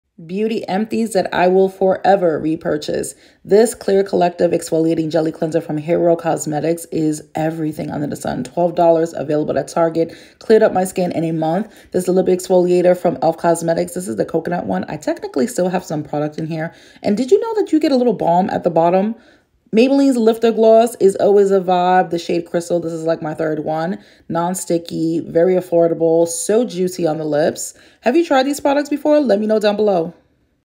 Beauty empties I will forever repurchase
Maybelline lifter gloss
elf cosmetics lip exfoliator
Hero cosmetics jelly cleanser